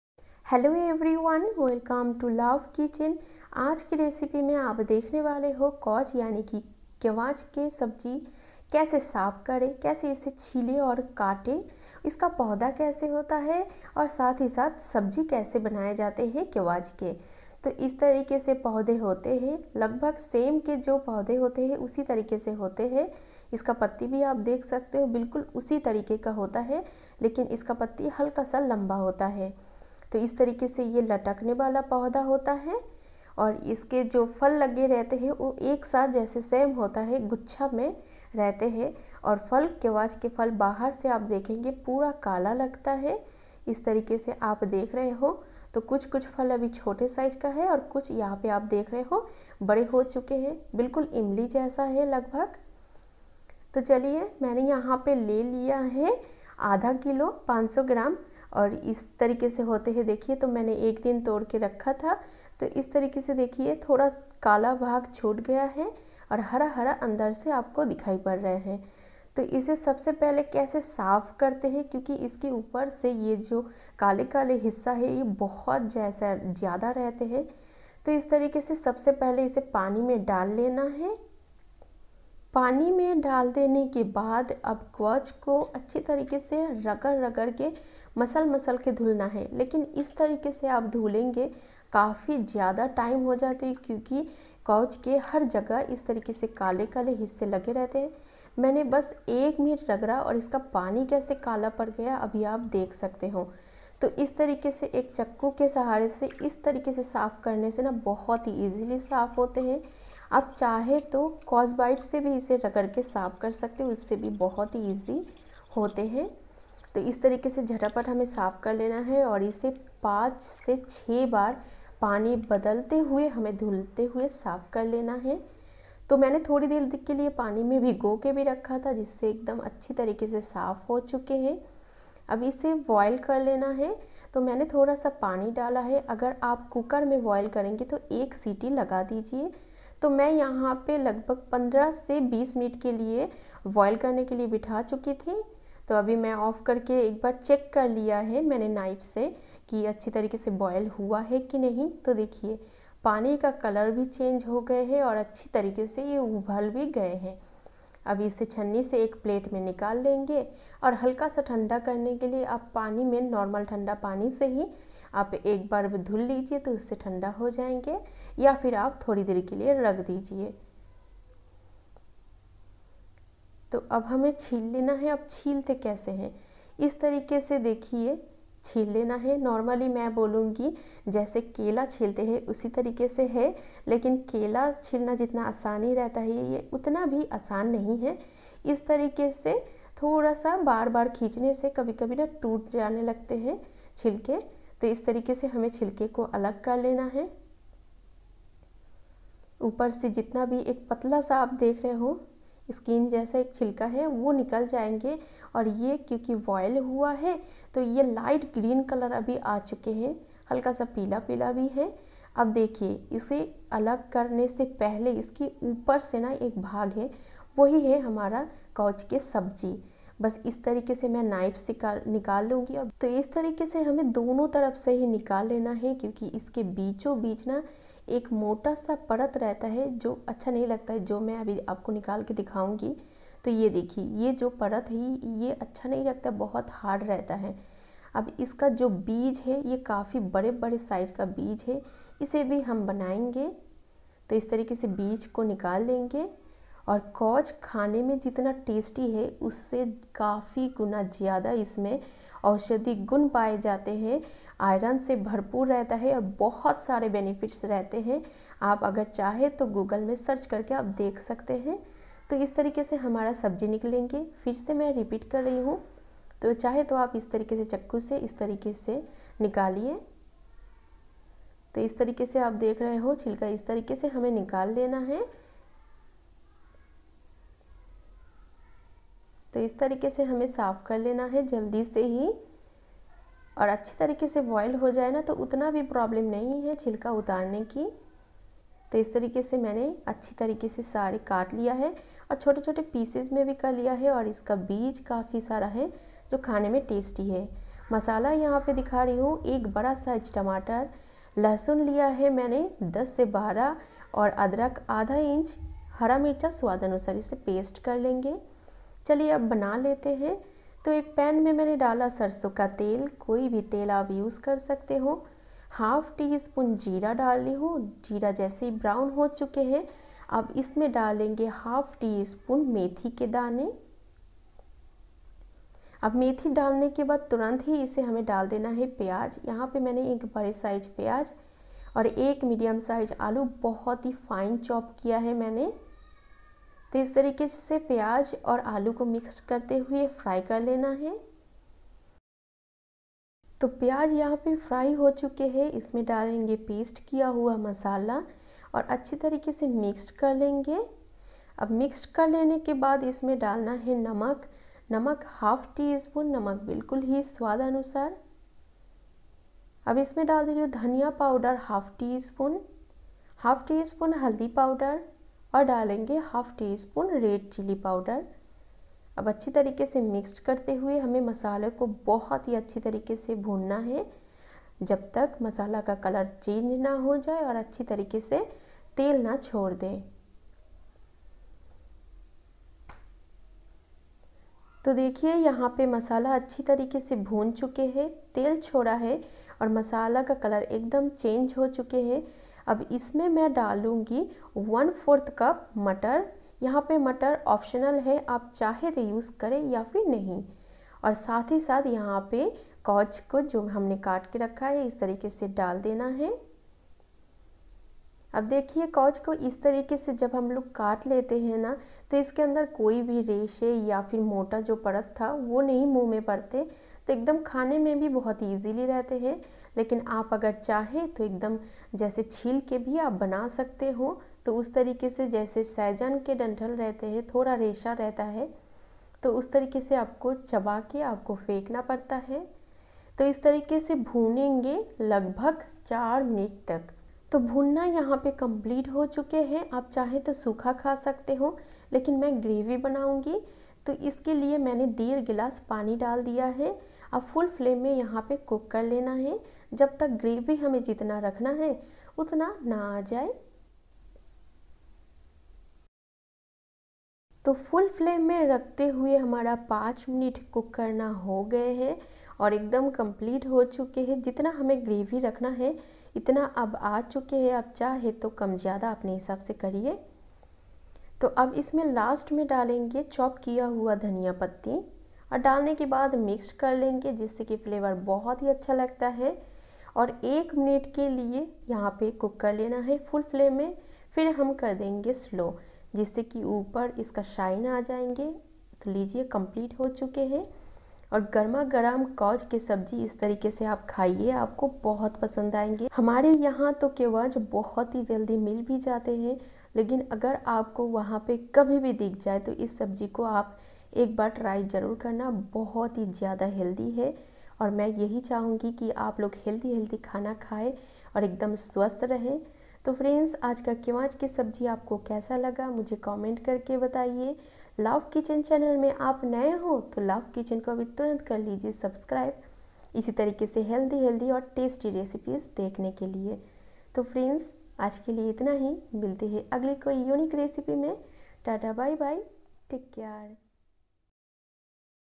ऐसे कौंच /केवांछ की सब्जी बनाकर खाएं स्वाद के साथ अपनी अंदरुनि शक्ति बढ़ाए | Kaunch Ki Sabzi recipe
कौंच / केवॉच / खुरसा की औषधीय सब्जी | Kevach , kauch , khursa ki sabji . Mucuna pruriens vegetable .

About : Hello friends welcome to all of you.  Today in Love Kitchen, we have prepared desi style after that vegetable Mucuna prurient / Kevach is very healthy and at the same time very tasty.